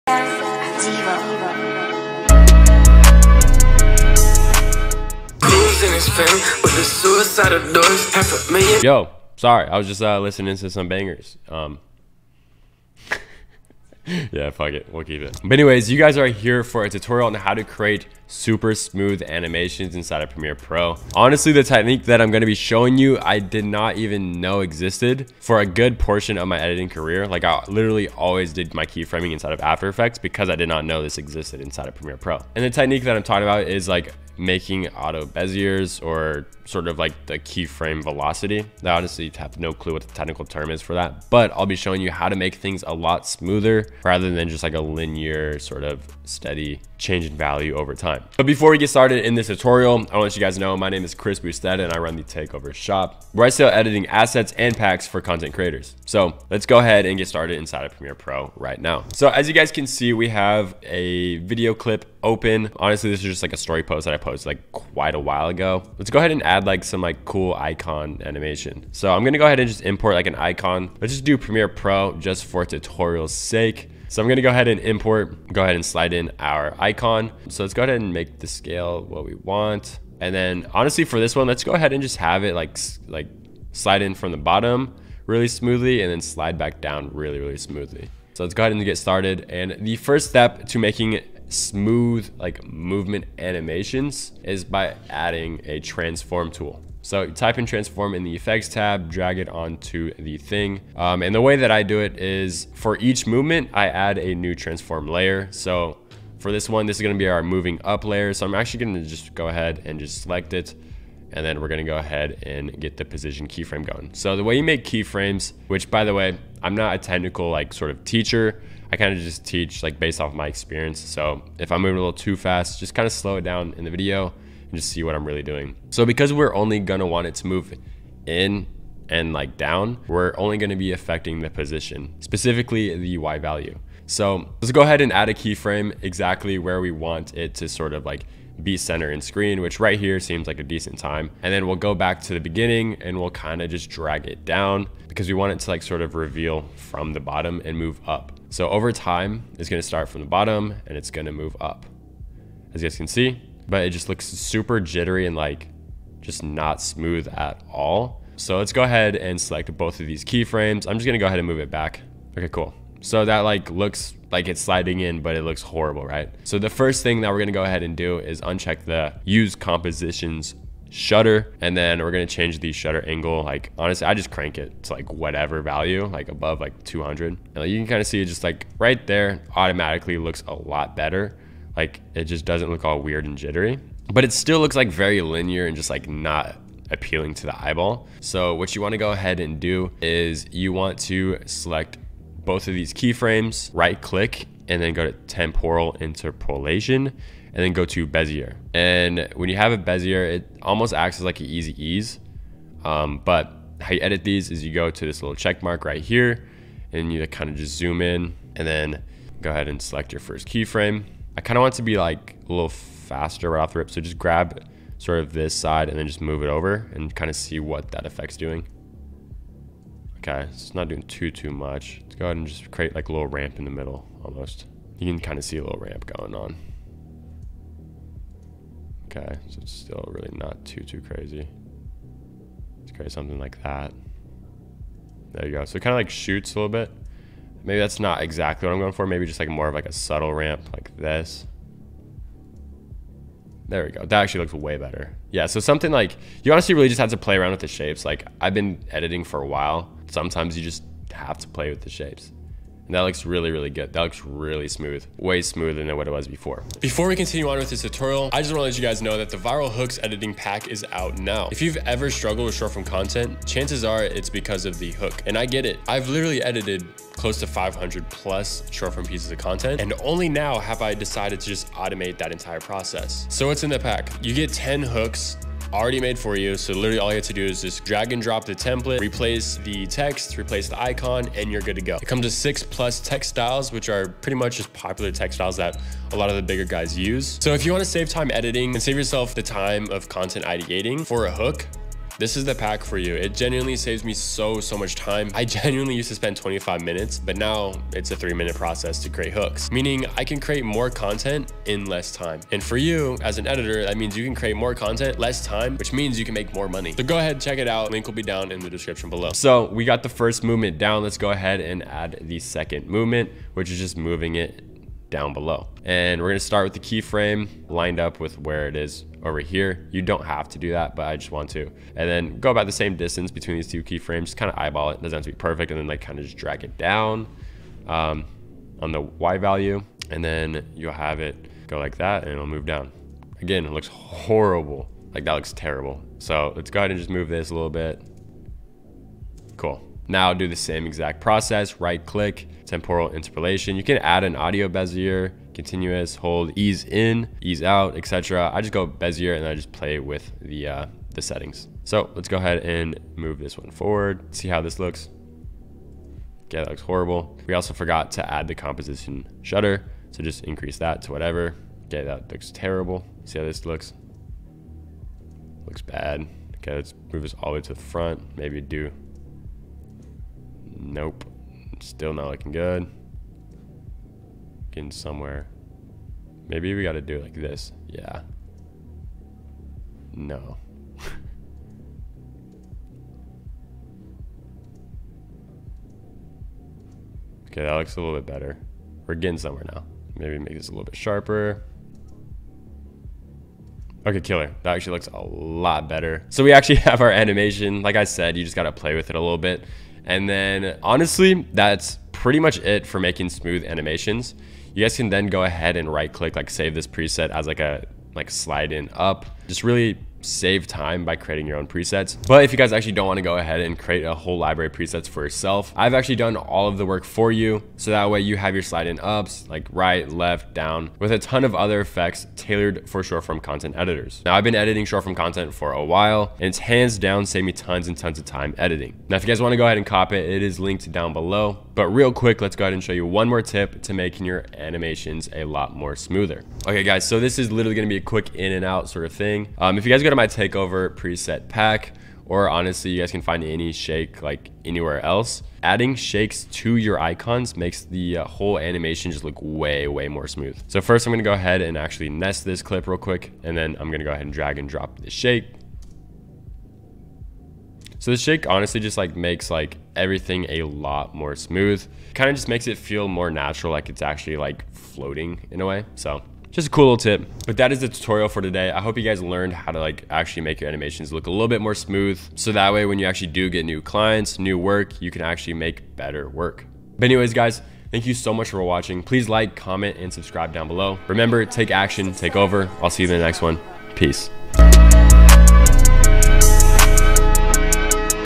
Master Animation In Premiere Pro! (SIMPLE Animations Tutorial)
Yo Creators/Video Editors! Today ill be teaching you have to create smooth animations in premiere pro. This is the basic building block of creating engaging video edits. I start off by opening premiere pro and importing our icon that we want to animate. Then that's when we decide to add a simple key framed movement "slide-in up". From there I show you have to add velocity to the movement, to add that realistic and smooth effect. Next, i show you how to add realistic motion blur to your animation. Once completed, you'll be able to create amazing, and smooth, simple animations.

Hope this editing tutorial was useful!

CODE: *CHRISS* for $5 off your order

Official Channel of Chris Boustedt